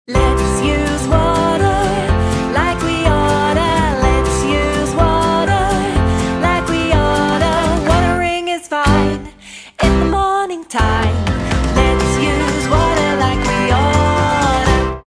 Let's use water like we oughta
Help us supply enough water to everyone this summer.  Morning time? Watering your garden is fine.  Please avoid watering between 3pm – 9pm.